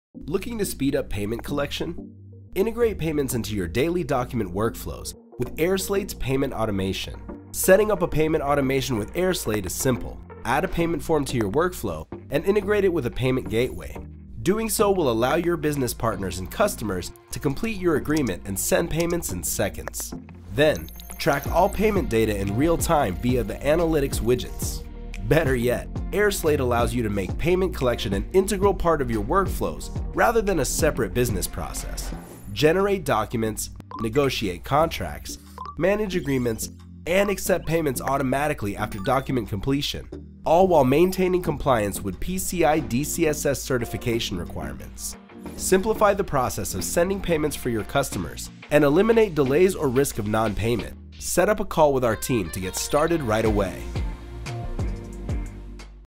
Collect Payments Automatically as Part of Your Document Workflow
Accept online payments quickly and securely. Build automated workflows with payment collection technology that allows your business partners and customers to complete agreements and send payments in seconds.

Automate real-time conditional routing and step assignments to set up and launch your streamlined workflows in minutes.

🔵 Partner with the most powerful business automation platform to delight your customers

🔵 Find everything you need to know about the latest business automation trends